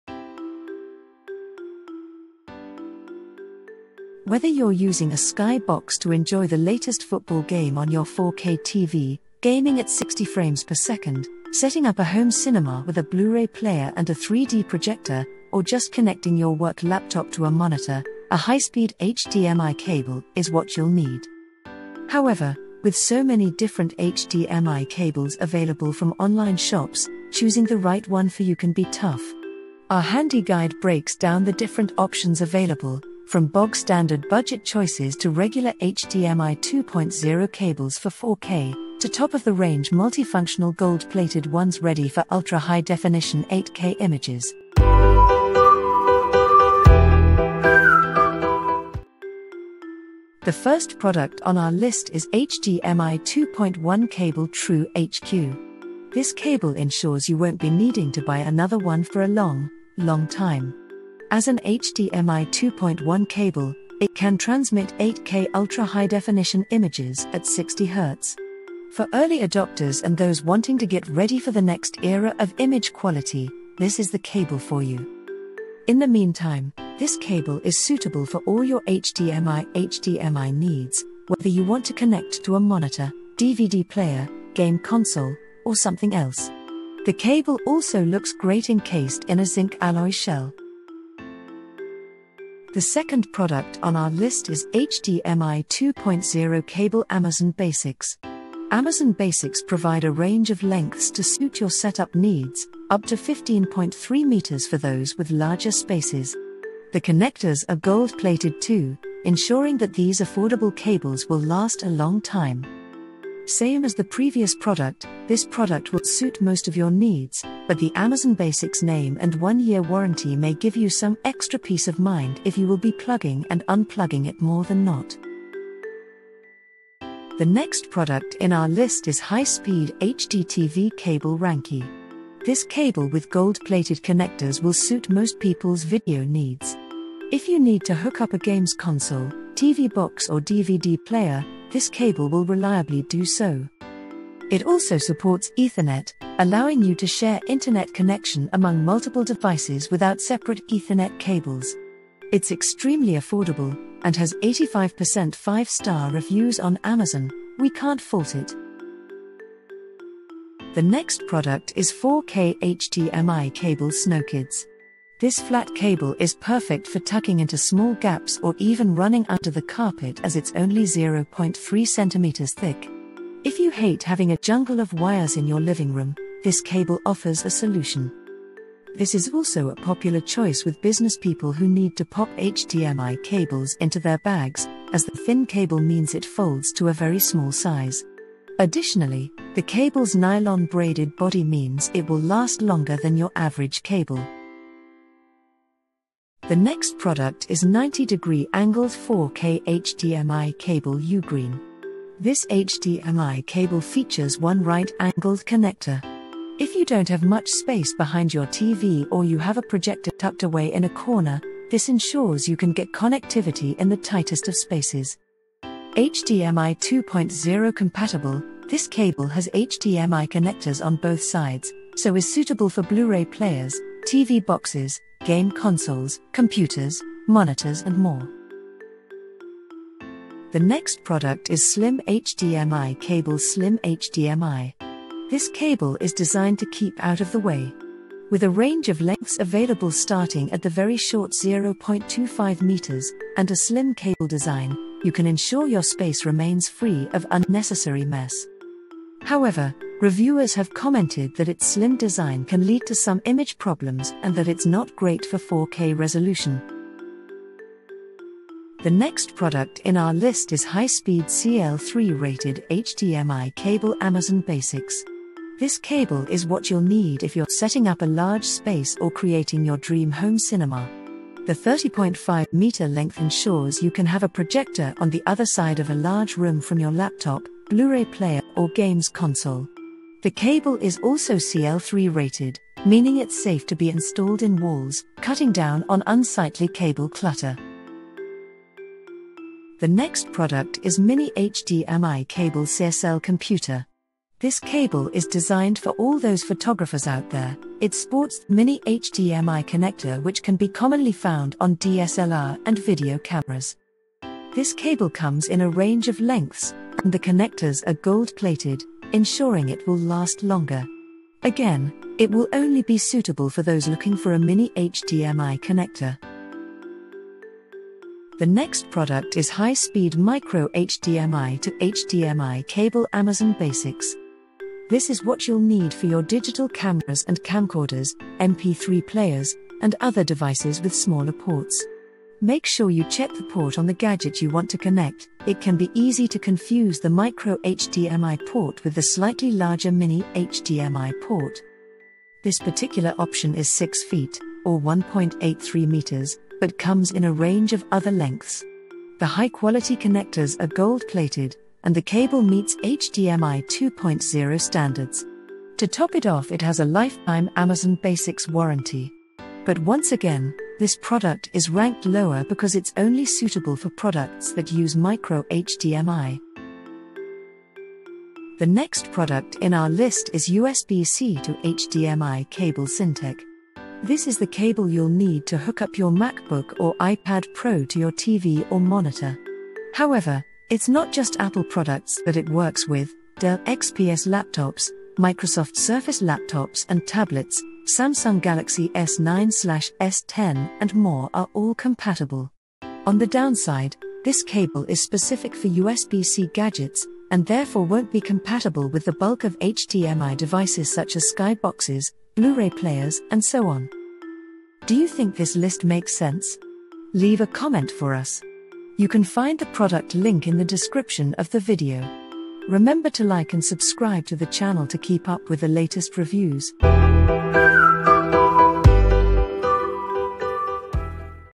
🌵 10 Best HDMI Cables 2022
00:00 Introduction
00:48 #1 - HDMI 2.1 Cable True HQ (£12.99)
01:30 #2 - HDMI 2.0 Cable AmazonBasics (£5.99)
02:05 #3 - High-Speed HDTV Cable Rankie (£5.99)
02:38 #4 - 4K HDMI cable Snowkids (£15.88)
03:14 #5 - 90 Degree Angled 4K HDMI Cable UGREEN (£7.99)
03:48 #6 - Slim HDMI Cable SlimHDMI (£9.99)
04:16 #7 - High-Speed CL3-Rated HDMI Cable AmazonBasics (£49.99)
04:47 #8 - Mini HDMI Cable CSL-Computer (£6.49)
05:16 #9 - High-Speed Micro HDMI to HDMI Cable Amazon Basics (£15.39)
06:06 #10 - USB C to HDMI Cable Syntech (£11.99)
06:43 Ending

Whether you’re using a Sky Box to enjoy the latest football game on your 4K TV, gaming at 60 frames per second, setting up a home cinema with a Blu-Ray player and a 3D projector, or just connecting your work laptop to a monitor, a high-speed HDMI cable is what you’ll need.
However, with so many different HDMI cables available from online shops, choosing the right one for you can be tough. Our handy guide breaks down the different options available, from bog-standard budget choices to regular HDMI 2.0 cables for 4K, to top of the range multi-functional gold-plated ones ready for ultra high definition 8K images.